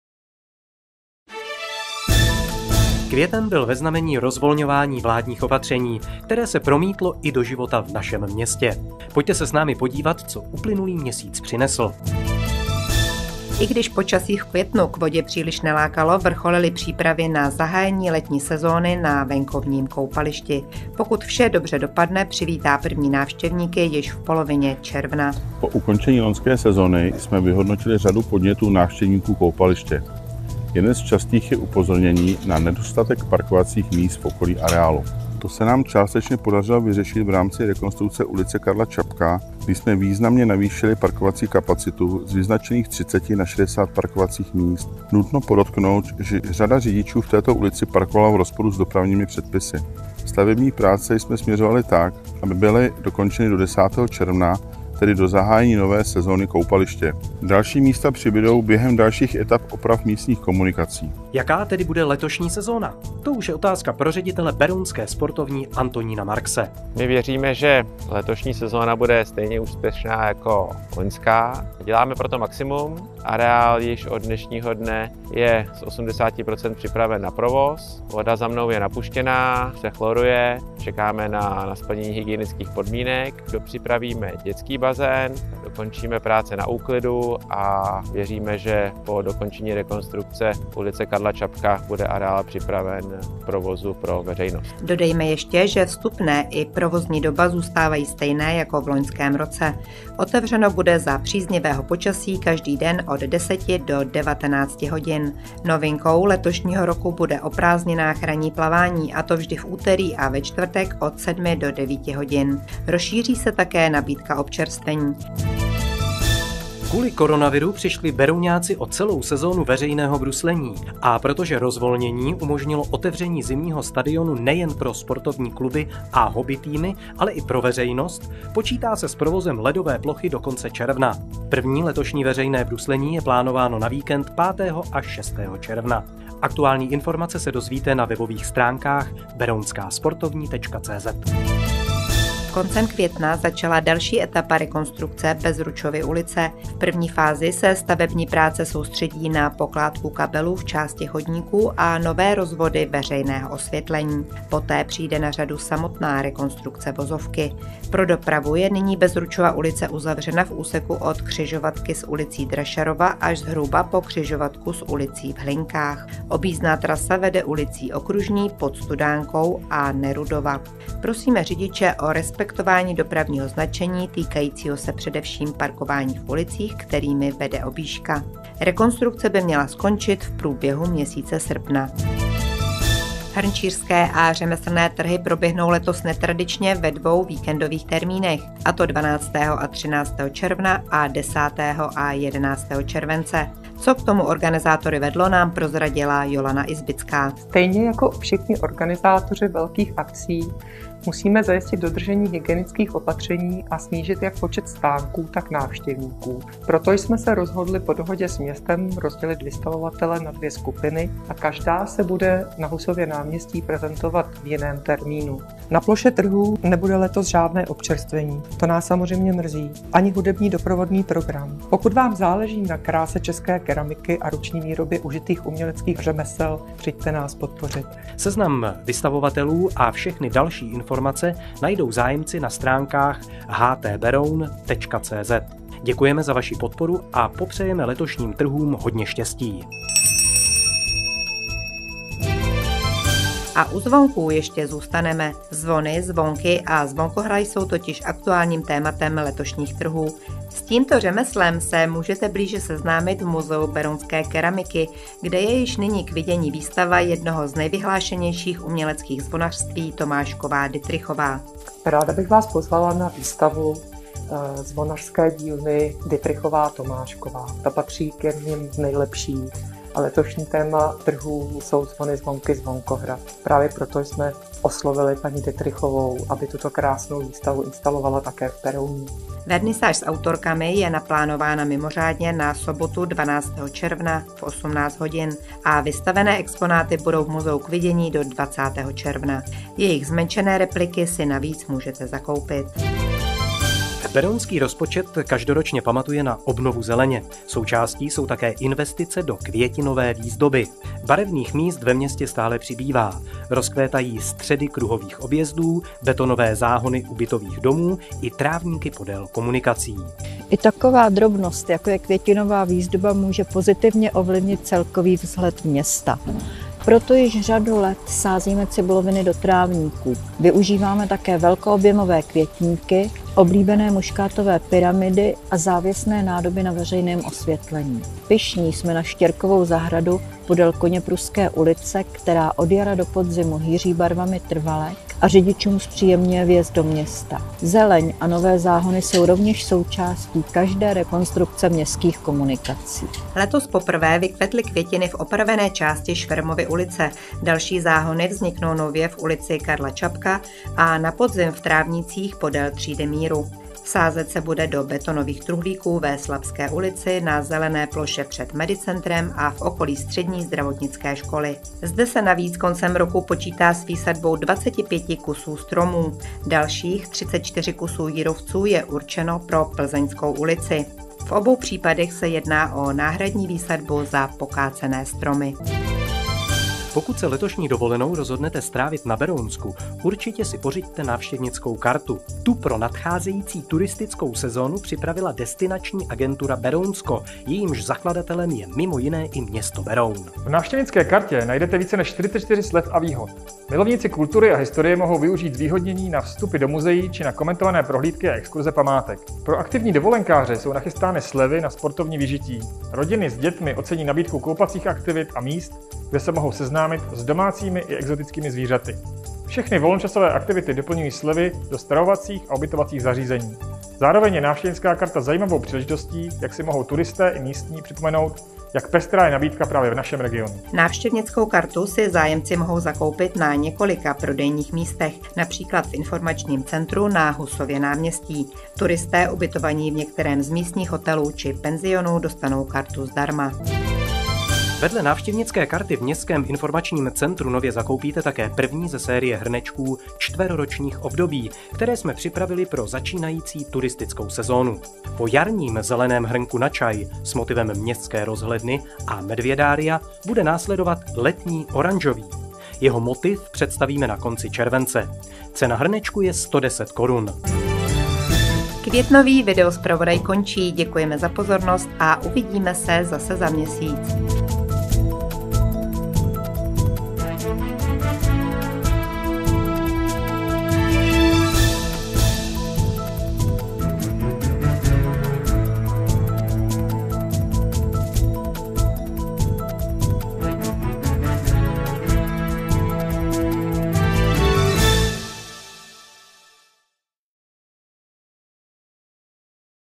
Berounský videozpravodaj 05/2021
V květnovém videozpravodaji najdete mimo jiné reportáže o zahájení sezóny na venkovním koupališti, o rekonstrukci ulic Karla Čapka a  Bezručova, dozvíte se, jak to bude letos s pořádáním oblíbených Hrnčířských a řemeslných trhů a nechybí tip na novinky, které si můžete zakoupit v informačním centru.